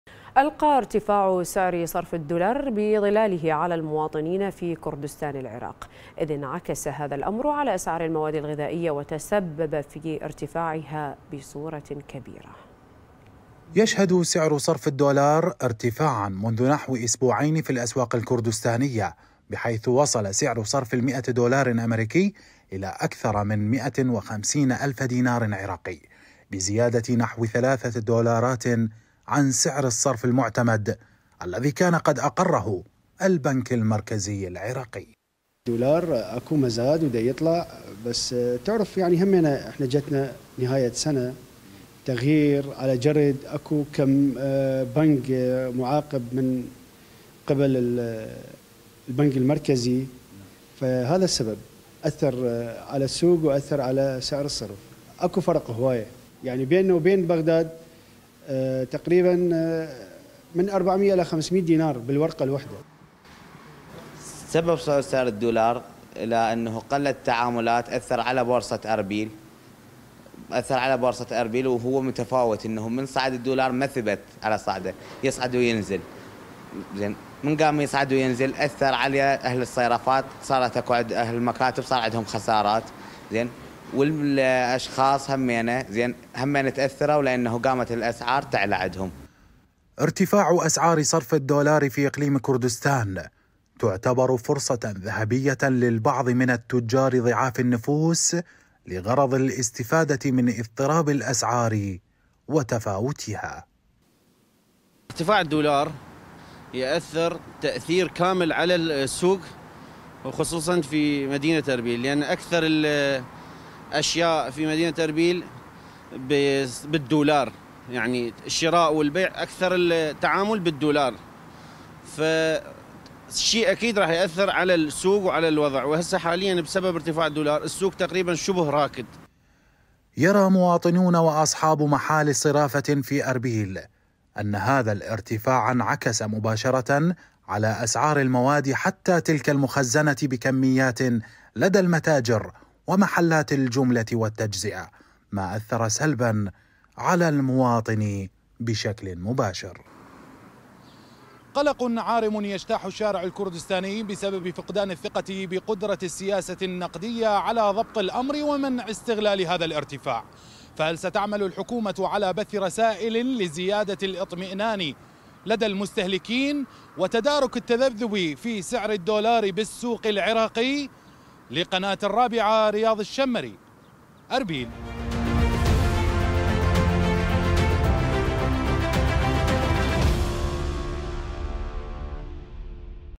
ارتفاع أسعار صرف الدولار يتسبب في غليان الأسعار بإقليم كردستان
تقرير: رياض الشمري